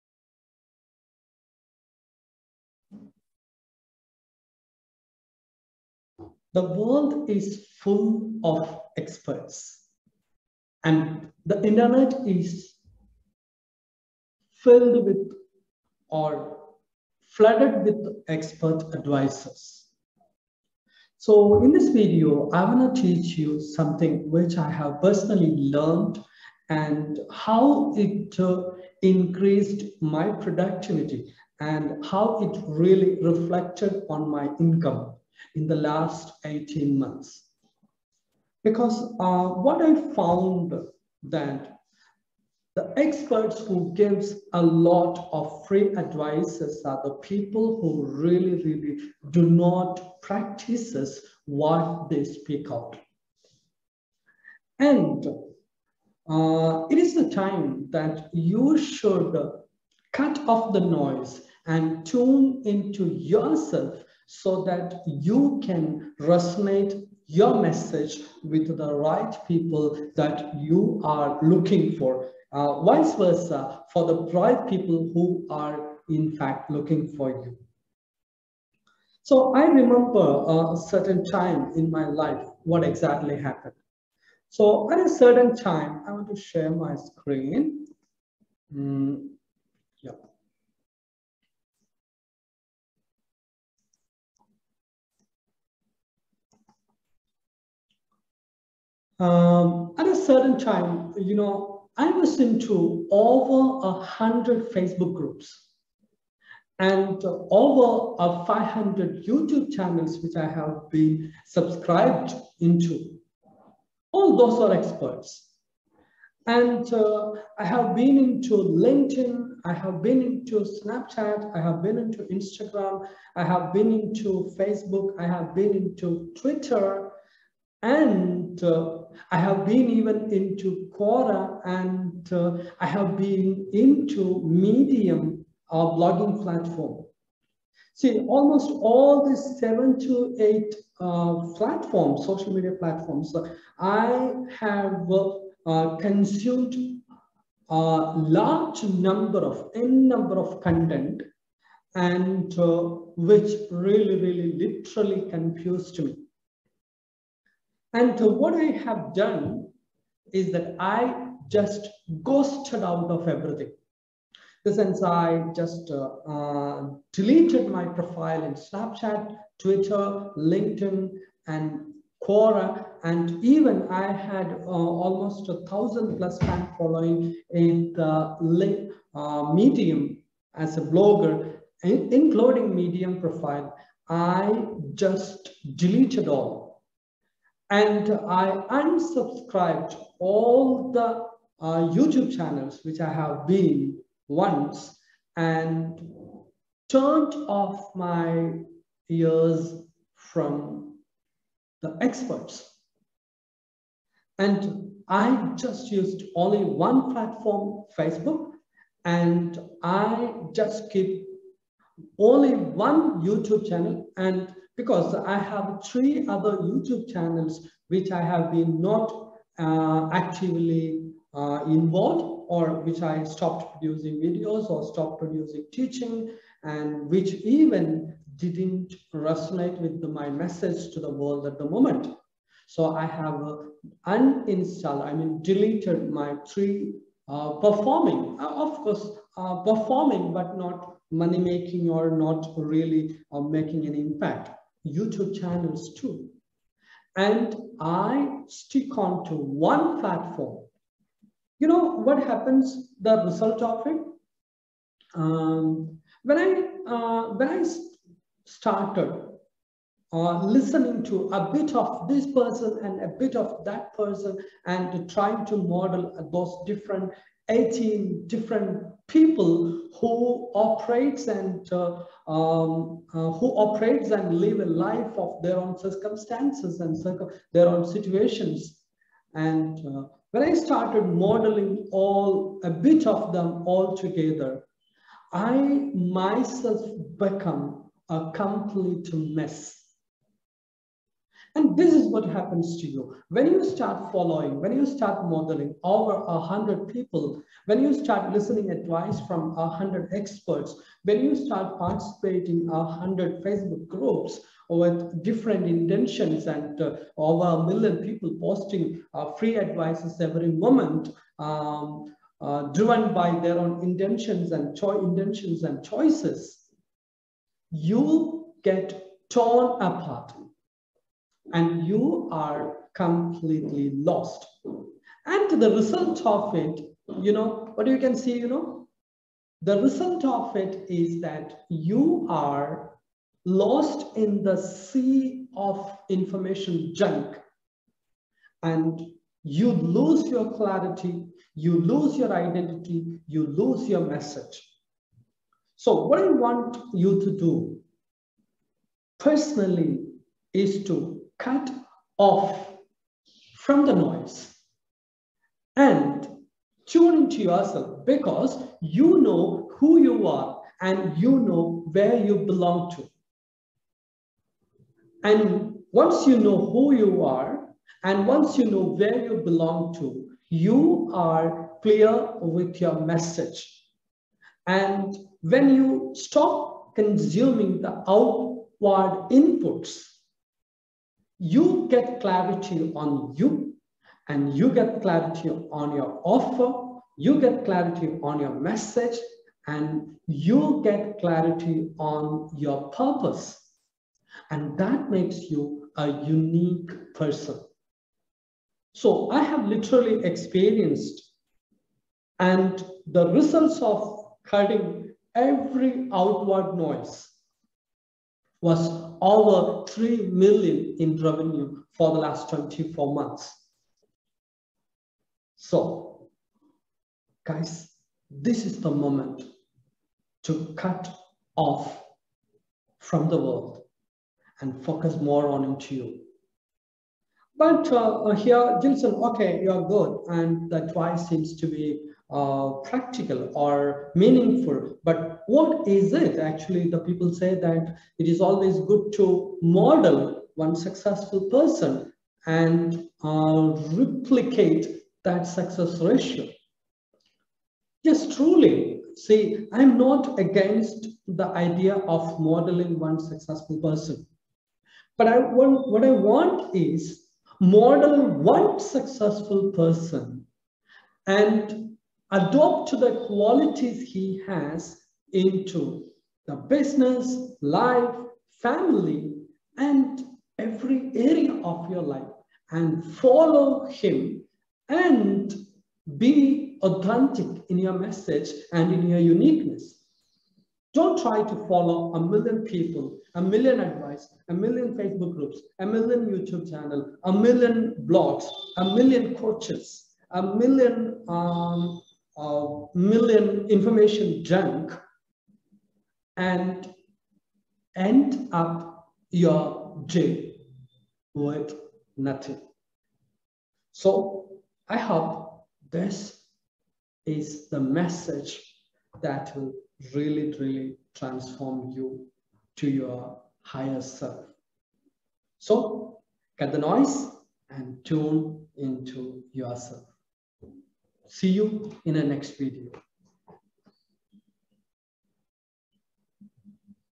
Cut the noise and Tune into yourself.
You know who you are and you know that you belong…
Then why to follow a million experts ?

Once I was too indulged in to the noise
I was in to over a hundred face book groups where over a a million posts content as expert advices.

And it took me almost two years to understand the effects it had on me and my life business and relationship

The world is flooded with “advice.”

Most of said advice is tendered by folks who aren’t actually doing what they say they’re doing and as such their advice will not work

Do you want to know what know what did I get cutting the noise and inputs?

Over 3 million in 24 months.

I knew who I am and I knew where I belong to, and ,it helped me to define my Niche crystal clear
It made me crystal clear in my message
It made me sell my products without even a sales call

Deleted
Quora
Medium blog with over thousand fan followers

Left over a 100 fb groups
Unsubscribed over 500 you tube channels
Unsubscribed over 1000 mailing lists ( Thanks to unrollme.com )
Deleted my own 3  you tube channels which I no longer post or which never resonate my messages
Unfriend over 100ds of great coaches and internet celebrities who sell expert advices free and paid.

When you are  trying to follow a “bit” of this person, a “bit” of that person, trying to model 18 different folks who all live and operate different ways and circumstances , Believe me , that you are messed up even before you realising it .

The Best Secret is to model someone who is successful.
Then your advise isn’t contradictory?
Yes folks I do believe it and in fact I do it

Find folks you align with philosophically in multiple areas.

 Business
 Family/people
 Energy/spirit

Align with only those few   And Cut the noise.
And In fact, if I ever make you feel small, belittled, or CONFUSED, you should unfollow me as well.

Cut the noise and customise
Your offer
Your messages
And your uniqueness.